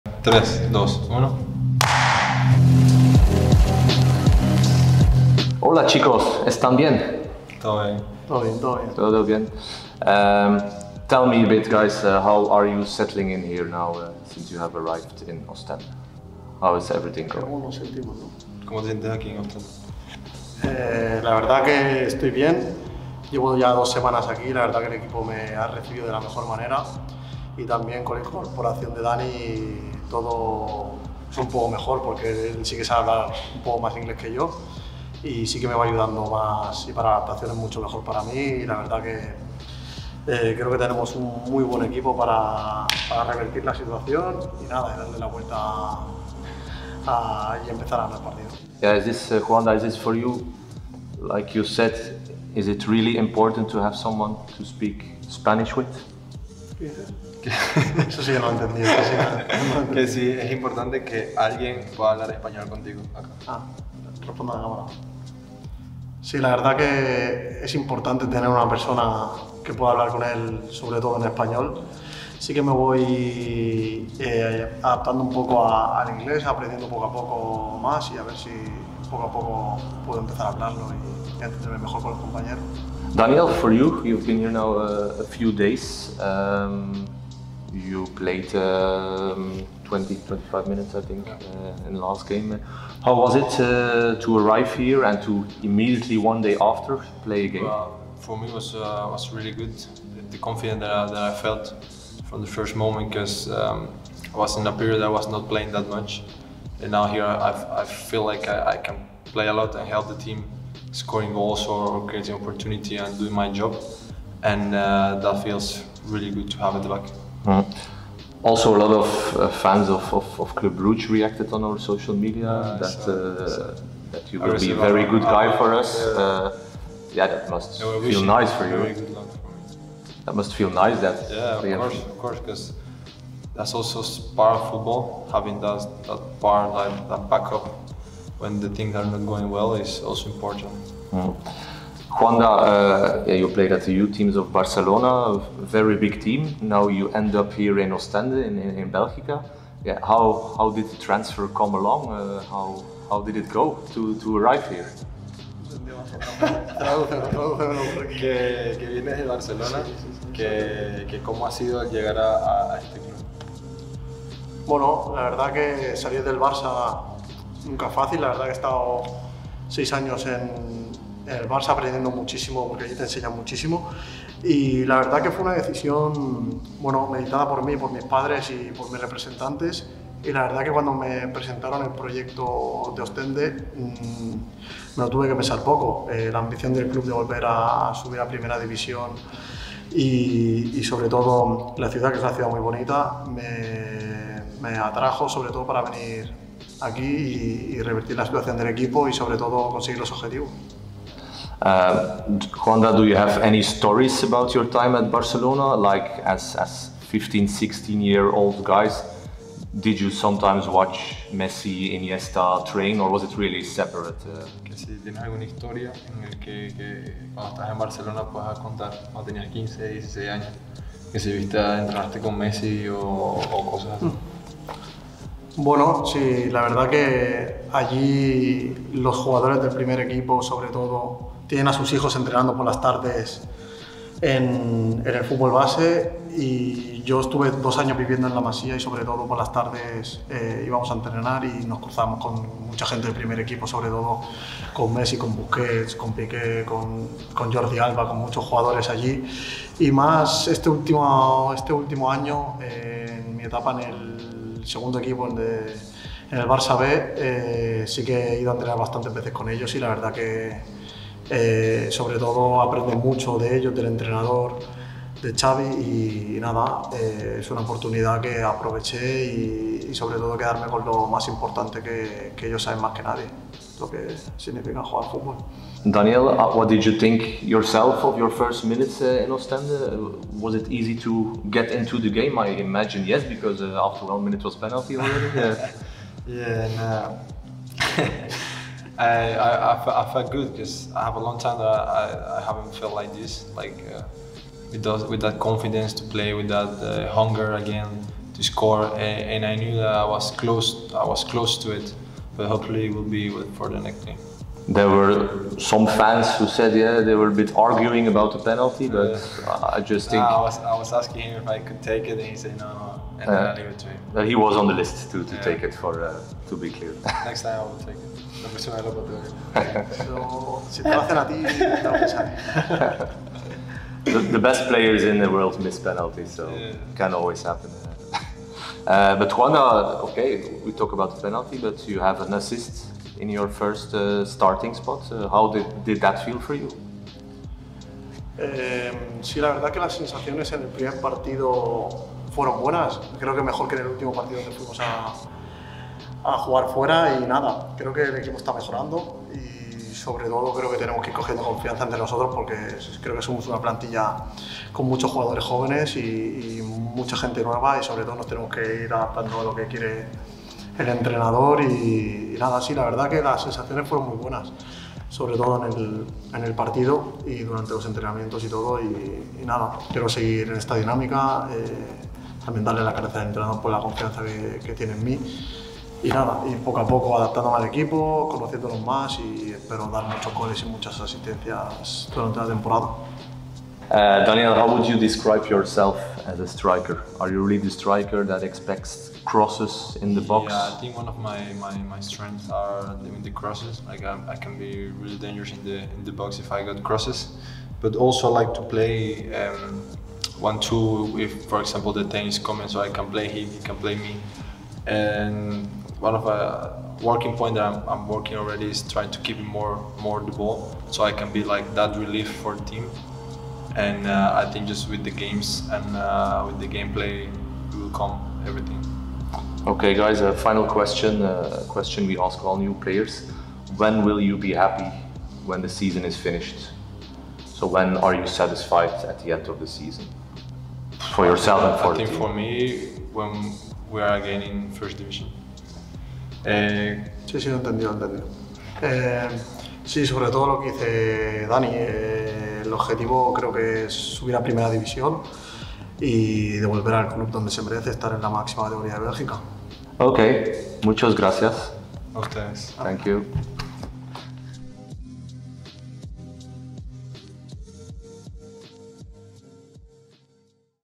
GET TO KNOW | Juanda Fuentes & Daniel Perez
Leer hier Juanda Fuentes en Dani Perez beter kennen! Ze hebben het onder meer over de taalbarrière, de strafschopsituatie van tegen Seraing en nog veel meer!

🎟️ 𝐒𝐄𝐀𝐑𝐂𝐇𝐈𝐍𝐆 𝐅𝐎𝐑 𝐓𝐈𝐂𝐊𝐄𝐓𝐒?